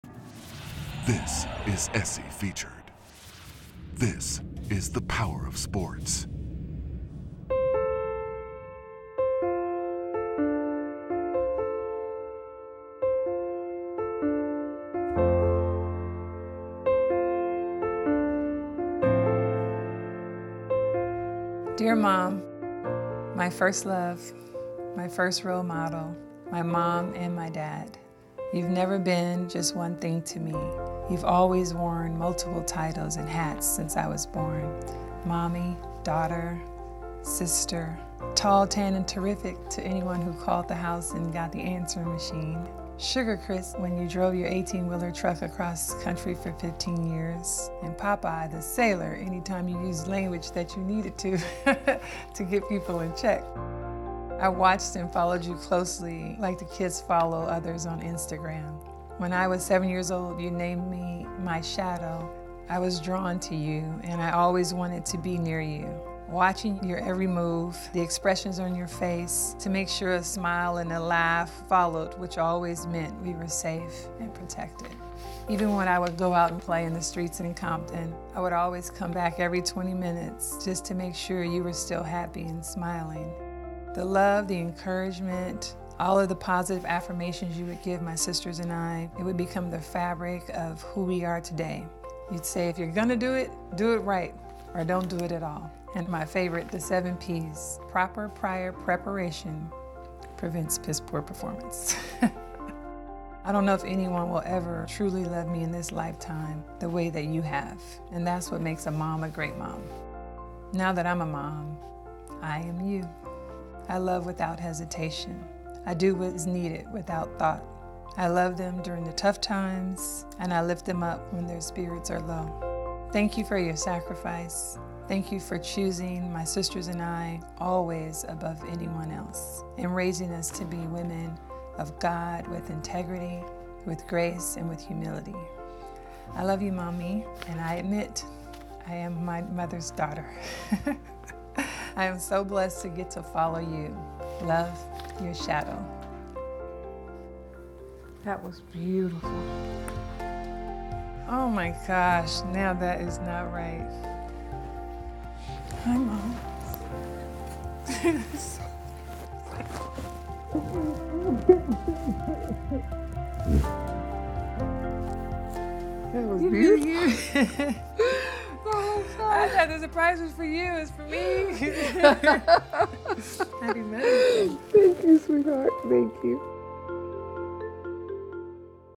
Lisa Leslie delivers heartfelt message to her mother | SC Featured
With her mother, Christine watching, WNBA legend Lisa Leslie reads her a heartfelt letter to describe how much she has meant to her and how it has helped Lisa become a mother as well. After Lisa is done reading, she gets a surprise as her mother walks into the room.